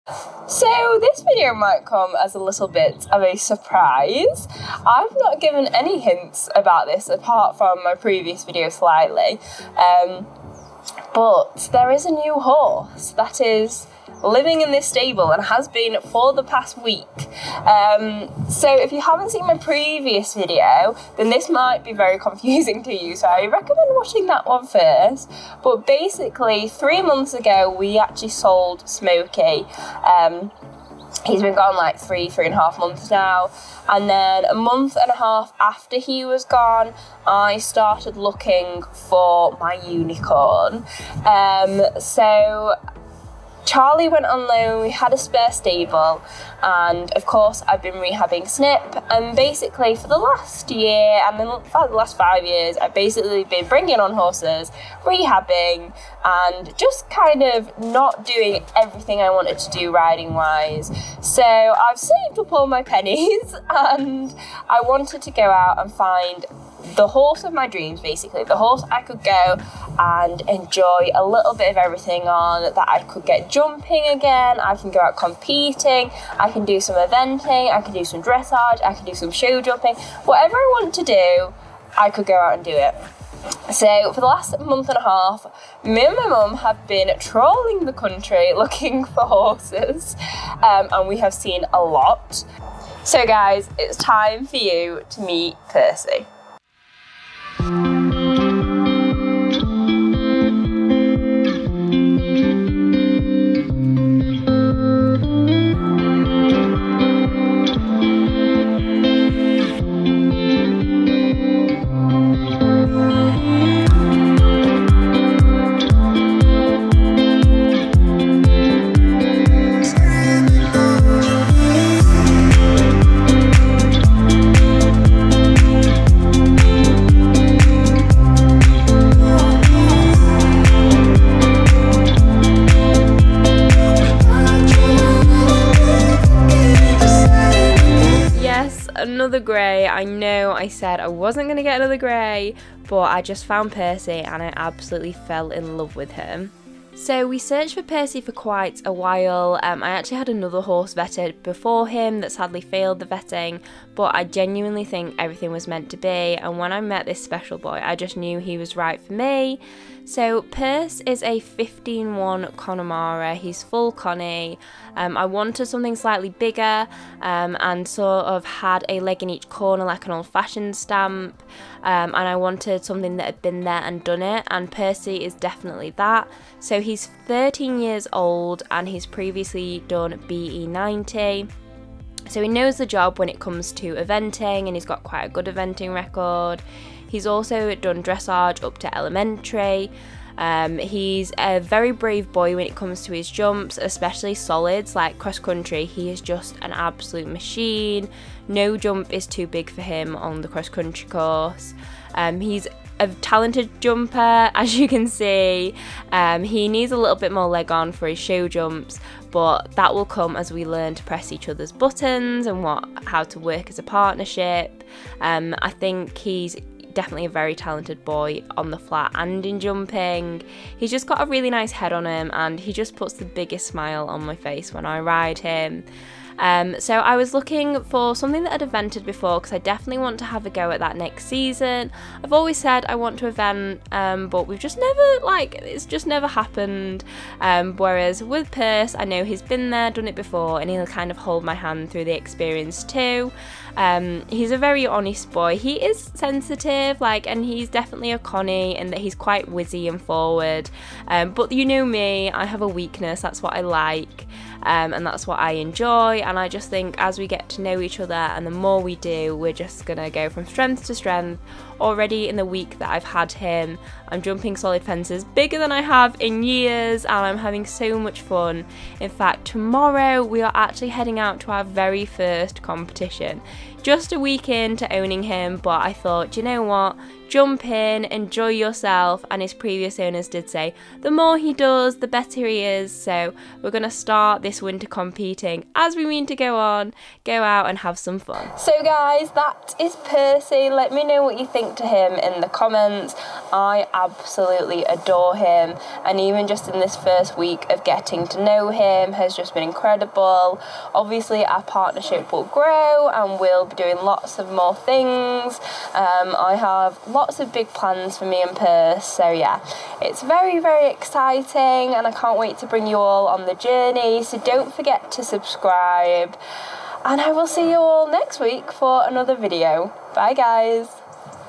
MEET MY NEW HORSE!!!
I bet you didn't see this one coming! I'm so excited to introduce you all to the latest member of the herd!

*Reuploaded due to the voiceover missing from the previous video so skip to 2:18 if you watched the last version to find out about my new addition